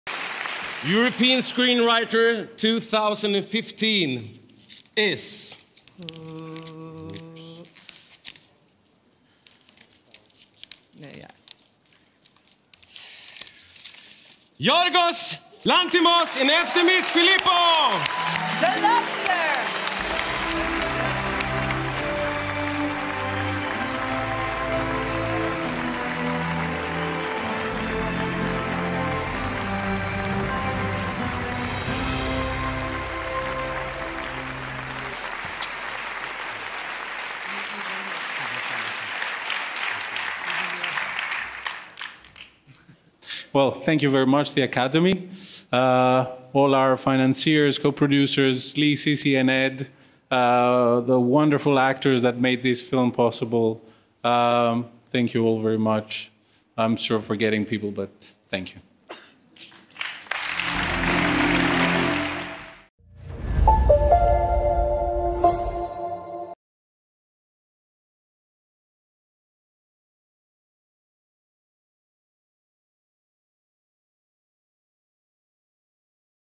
Yorgos Lanthimos & Efthimis Filippou - European Screenwriters 2015 for THE LOBSTER
Yorgos Lanthimos & Efthimis Filippou are European Screenwriters 2015 for THE LOBSTER at the 28th European Film Awards in Berlin, Germany.